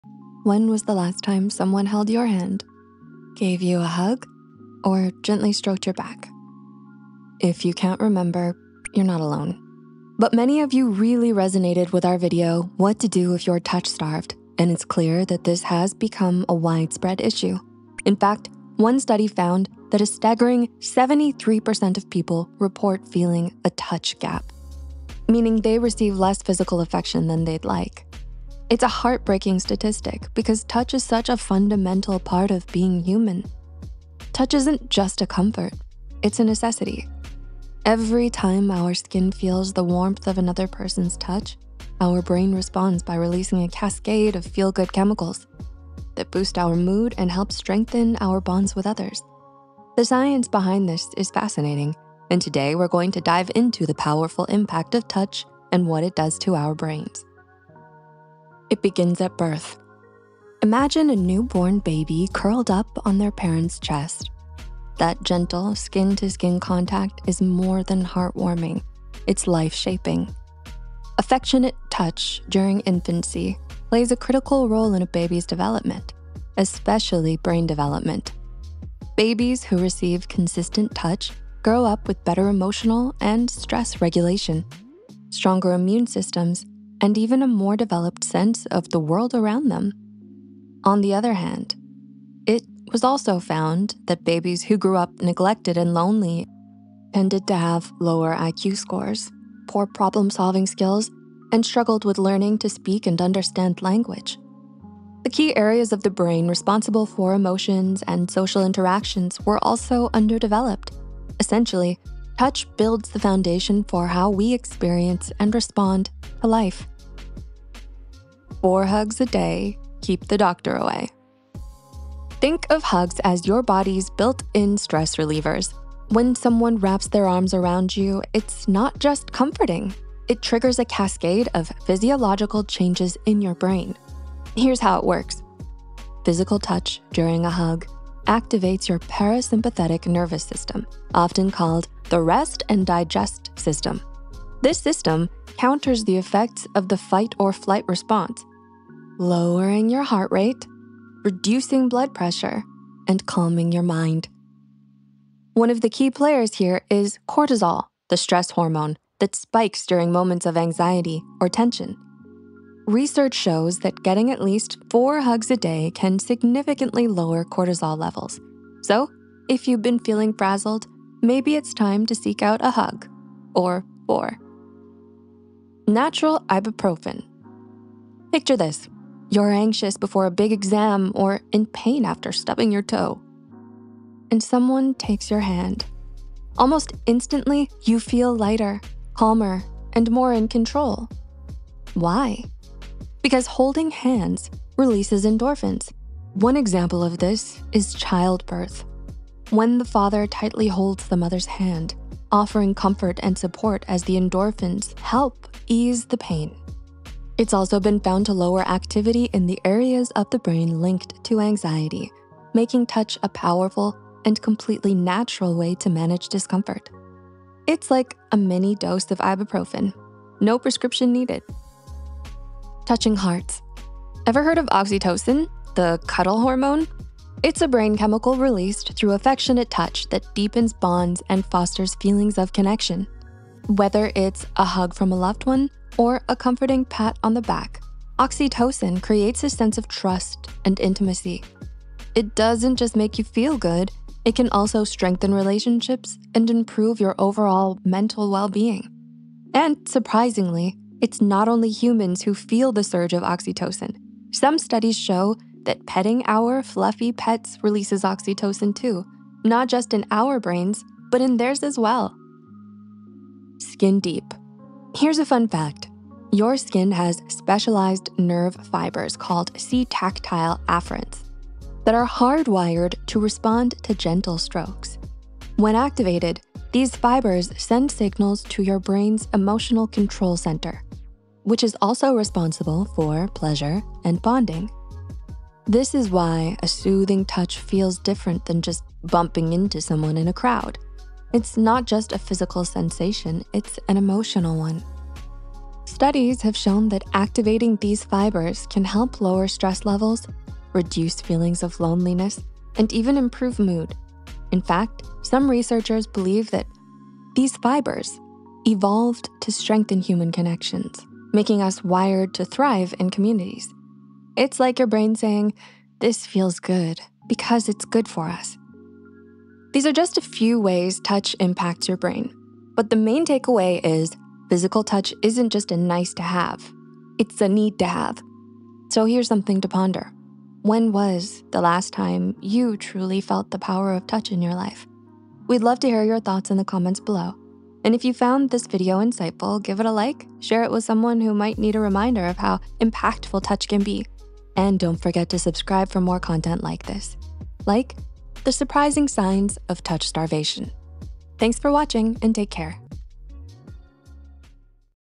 What Physical Touch Does To Your Brain
When was the last time you felt the power of touch? Whether it’s a hug, a pat on the back, or holding hands, physical touch isn’t just comforting—it’s essential for our well-being. In fact, studies show that 73% of people feel a “touch gap,” craving more physical affection in their lives.

In this video, we explore the fascinating science behind touch and its profound impact on your brain and mental health:

✨ **The Role of Touch in Childhood Development:** How early affection shapes emotional and cognitive growth.
🤗 **The Healing Power of Hugs:** Why four hugs a day can reduce stress and lower cortisol levels.
👫 **Natural Pain Relief:** How hand-holding can release endorphins and ease anxiety.
💓 **Oxytocin and Connection:** The "cuddle hormone" that strengthens bonds with loved ones—and even pets!
🧠 **Your Skin’s Special Touch Fibers:** The nerve fibers designed to make gentle touch emotionally rewarding.

Physical touch isn’t just a luxury; it’s a human need. From boosting mood to lowering stress, its benefits are undeniable. So, when was the last time you truly felt connected through touch?

**Related Content:**
- *What to Do If You’re Touch Starved*
- *The Surprising Signs of Touch Starvation*

**References:**
- Beßler, R., et al. (2019). The “Longing for Interpersonal Touch Picture Questionnaire.” International Journal of Psychology.
- Coan, J. A., et al. (2006). Lending a Hand: Social Regulation of the Neural Response to Threat. Psychological Science.
- Goldstein, P., et al. (2018). Brain-to-Brain Coupling During Handholding is Associated with Pain Reduction. Proceedings of the National Academy of Sciences.
- Vinmec.com. (2024). How Many Hugs Do We Need a Day?

Team:

Writer: Stela Košić
Script Editor & Manager: Chloe Avenasa & Kelly Soong
Animator: Zuzia
YouTube Manager: Cindy Cheong